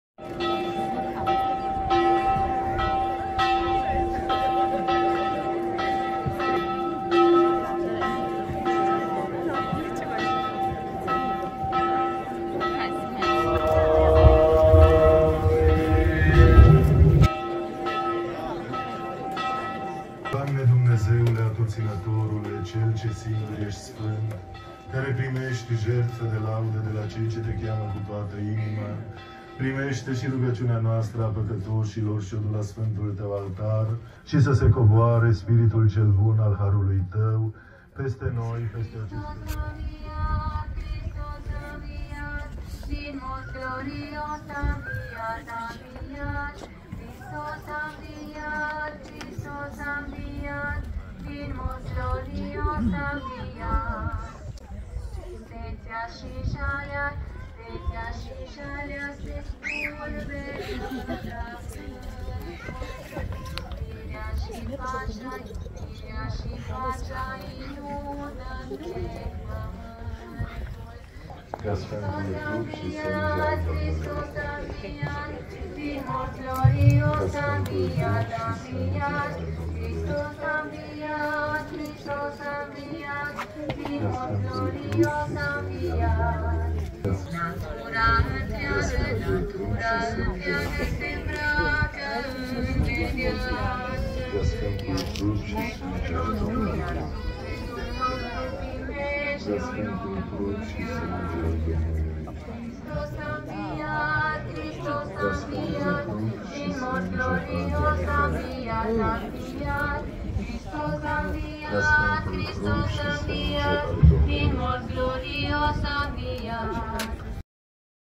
Easter Sunday at a woodenchurch of Maramures
Easter Sunday - April 16, 2023. Wooden church of woodenchurch of Surdesti, Maramures, Romania.

Spending Easter in a village of Maramures (nort-west of Romania) is like travelling back in time. You are going to see crowded churches, the strong faith being shared by old and young alike.
You are going also to rediscover old traditions. You meet men and women, girls and boys dressed in traditional costumes, some of them handed over from their grandparents. And they dress these hand-woven cloths in the most natural way possible, because...